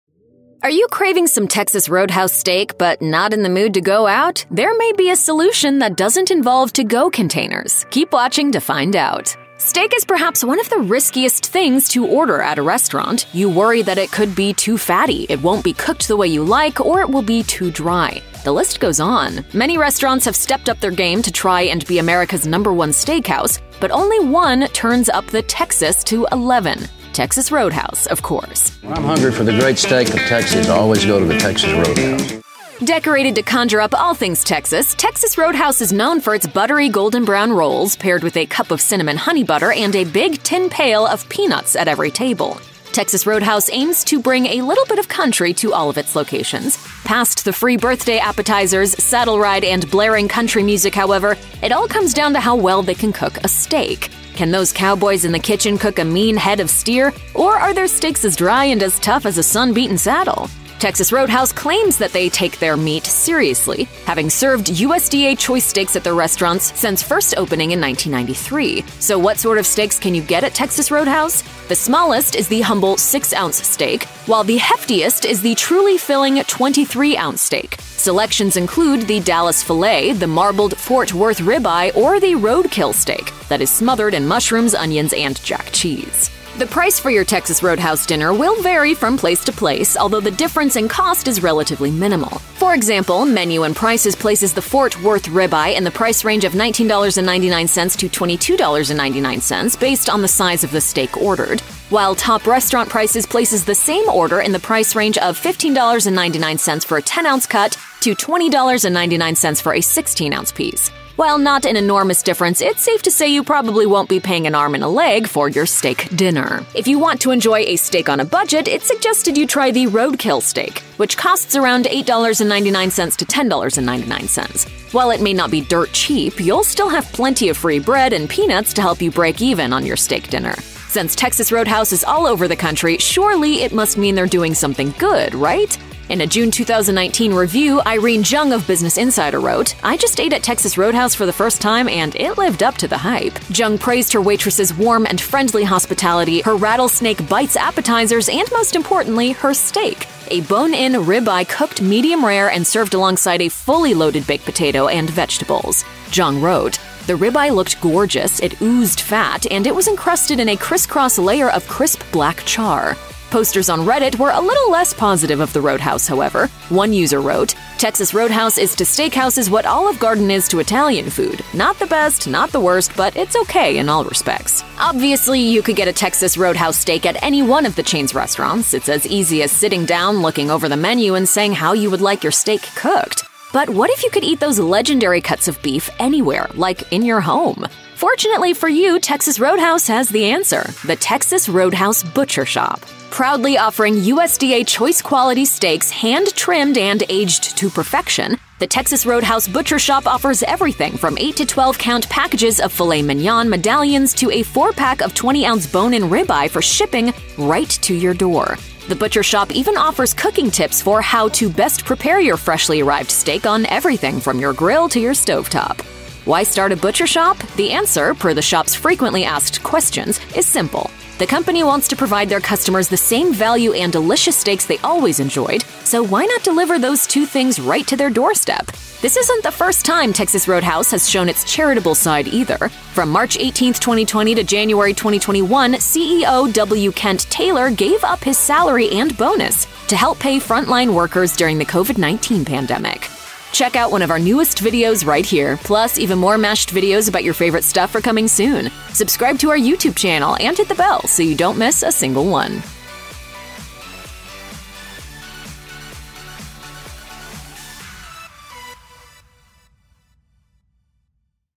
What To Know Before Eating Texas Roadhouse Steak Again
Since 1993, Texas Roadhouse has served only USDA Choice steaks alongside its other popular Texas-sized portions of tasty fare like free peanuts and golden-brown rolls served with cinnamon honey butter. Yum! Texas Roadhouse aims to bring a little bit of country to all of its locations, which adds to the charm. Past the free birthday appetizers, saddle rides, and blaring country music, however, it all comes down to how well they can cook a steak. Looking for a refresher on the steakhouse chain and what it has to offer, especially in the steak department? Here's what to know before eating Texas Roadhouse steak again.